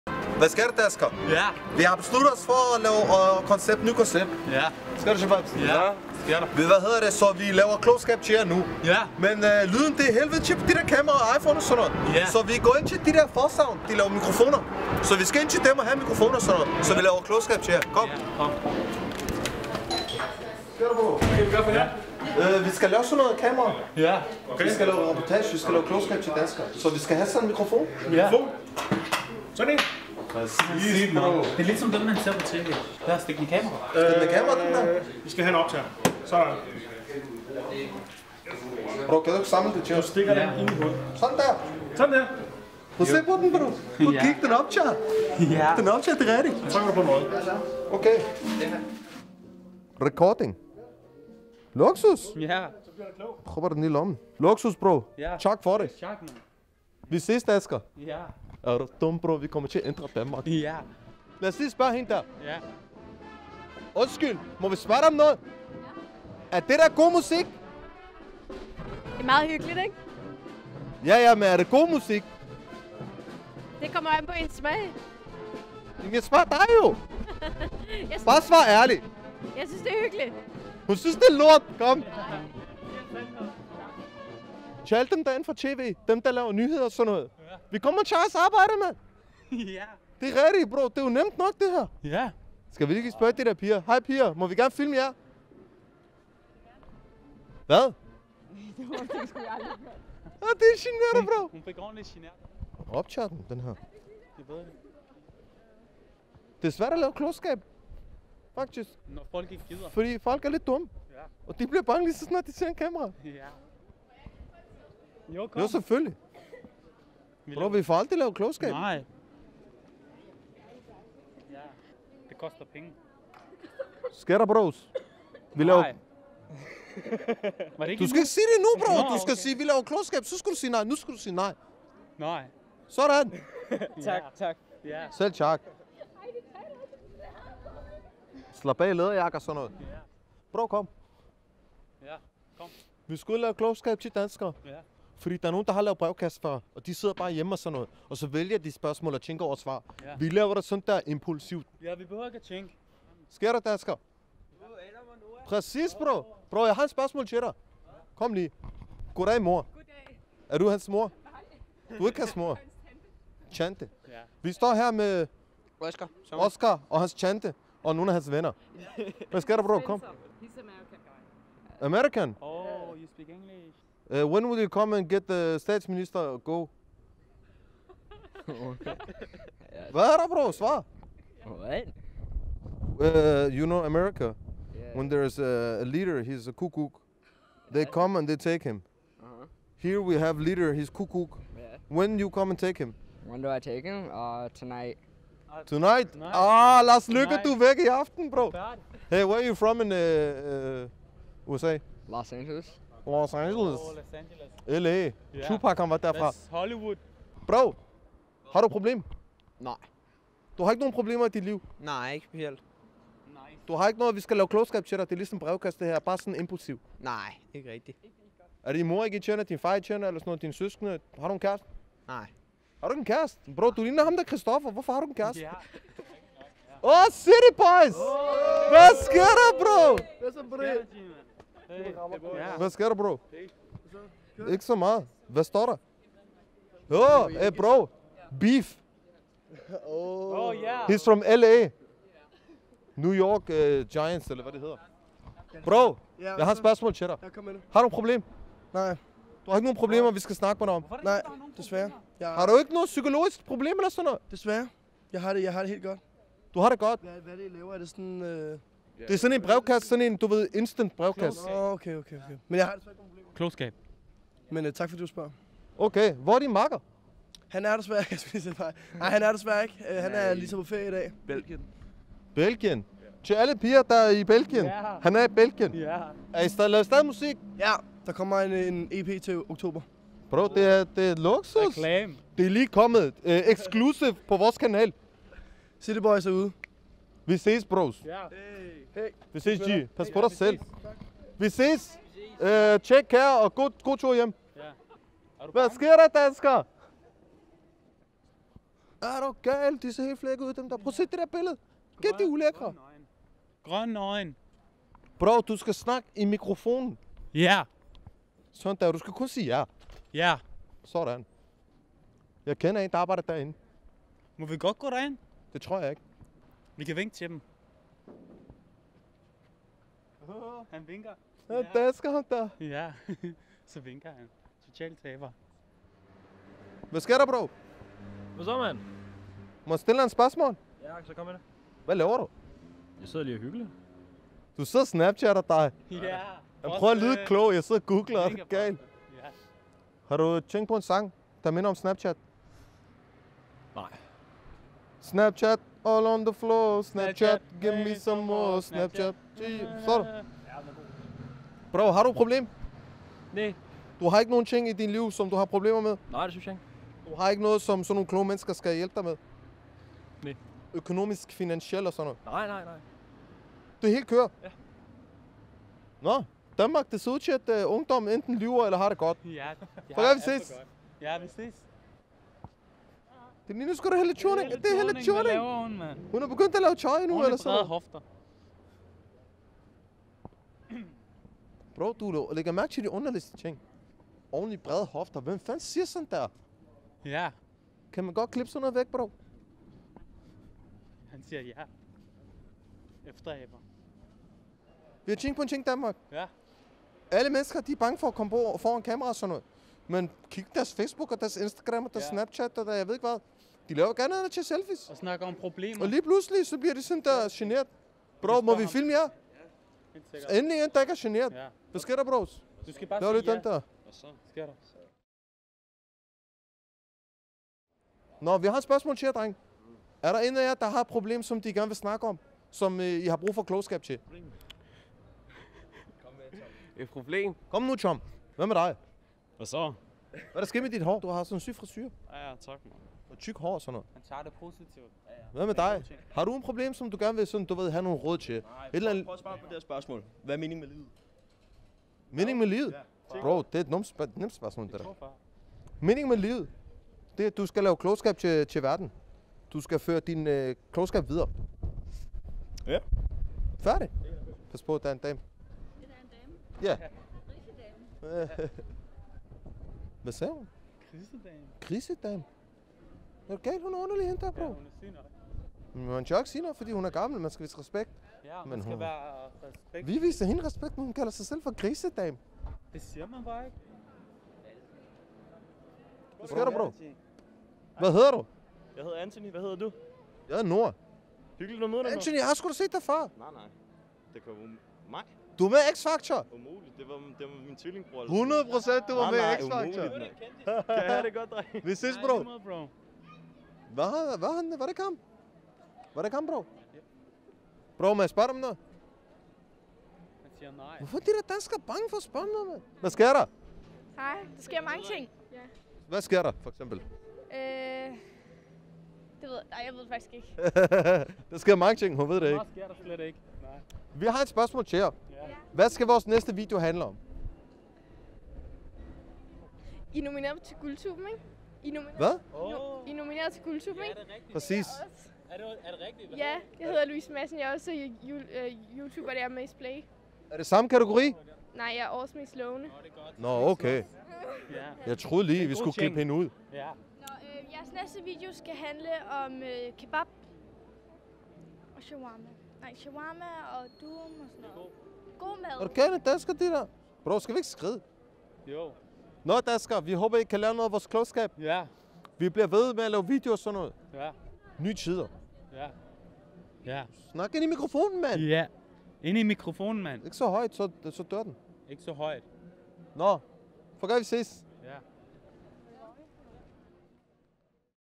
Adam & Noah laver klogskab til danskere!
Hva sker der danskere?!?!?! Det er nye tider! Vi laver klogskab til jer på gaden fordi det mangler der i DK. Så værsgo! Og det var så lidt :)